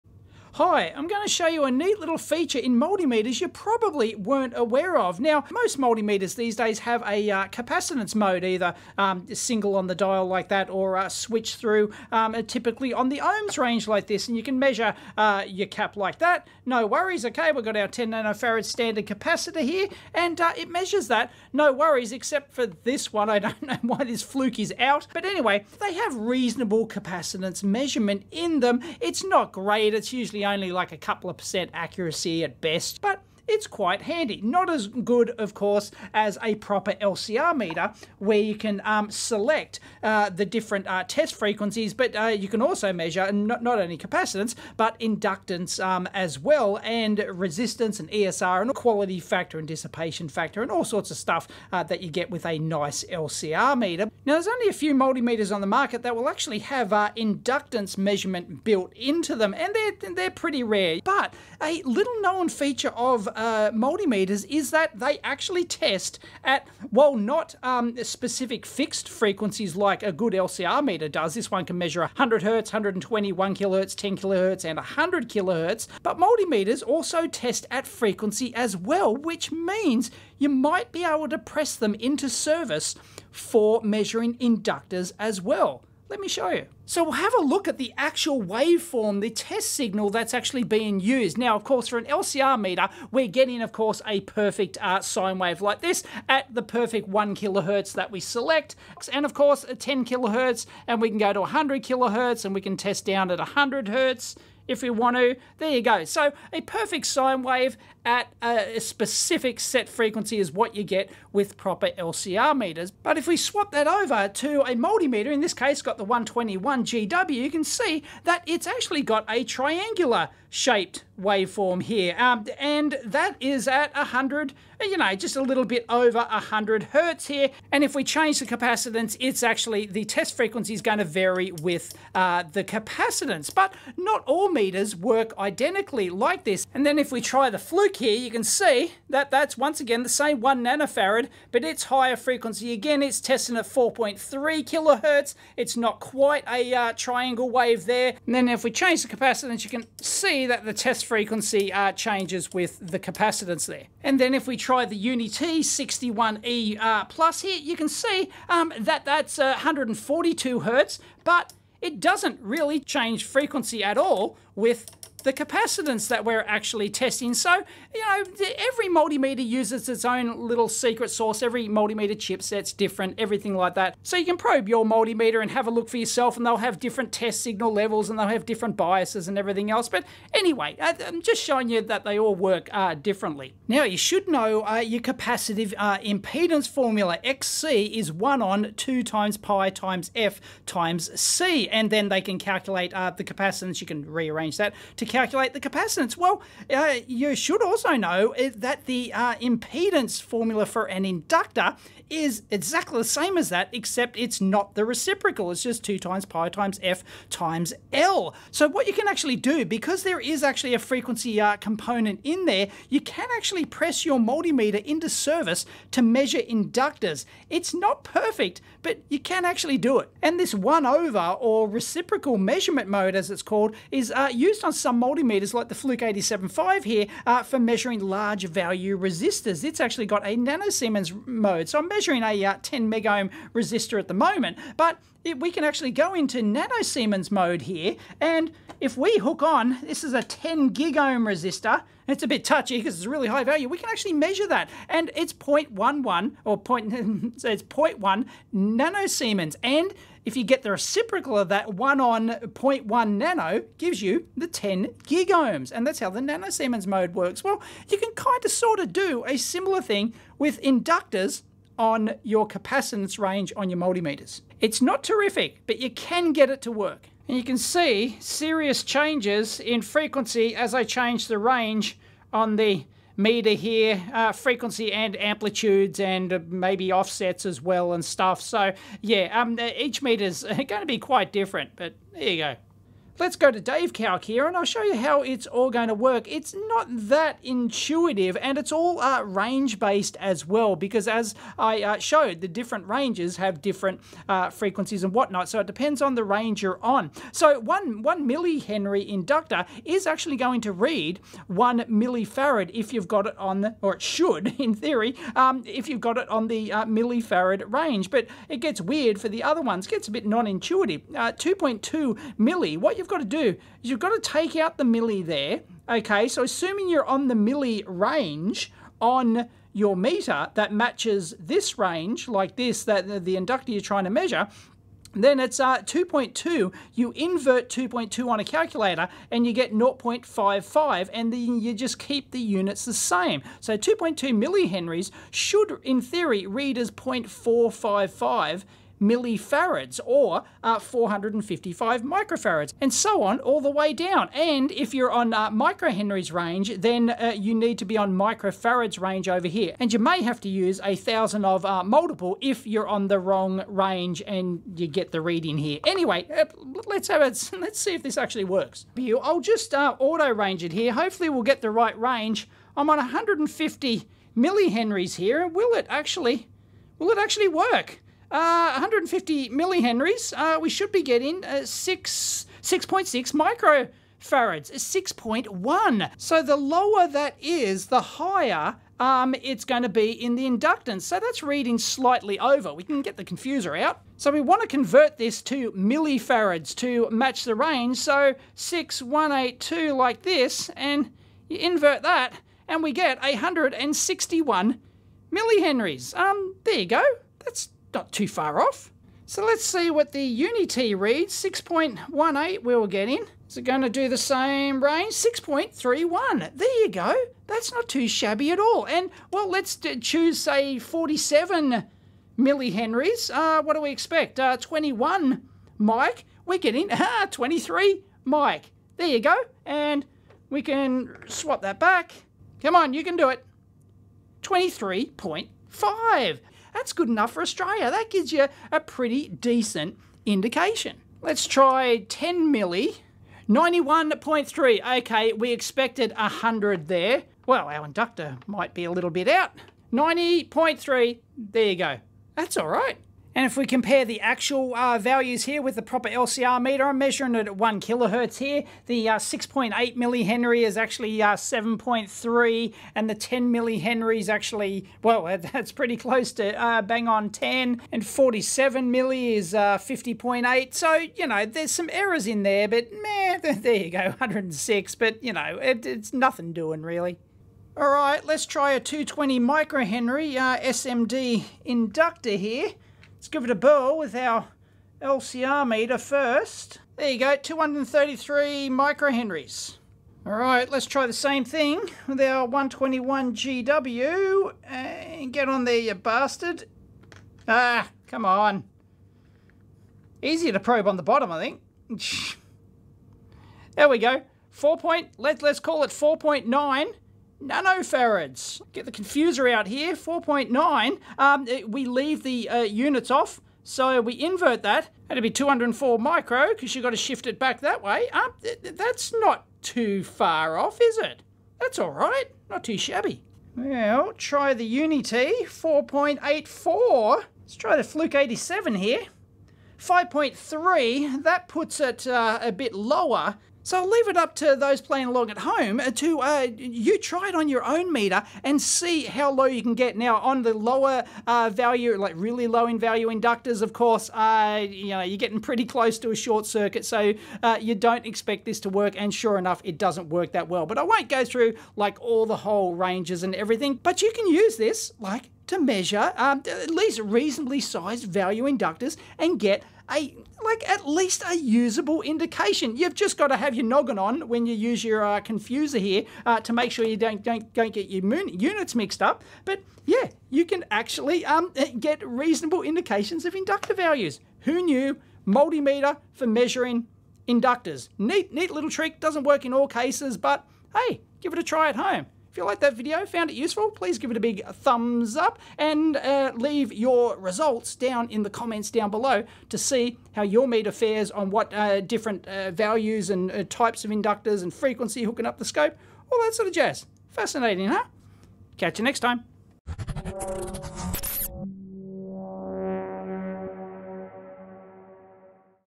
EEVblog 1465 - Your Multimeter Can Measure Inductors
Handy tip on how you can use your multimeters capacitance range to measure inductors, using the reciprocal method.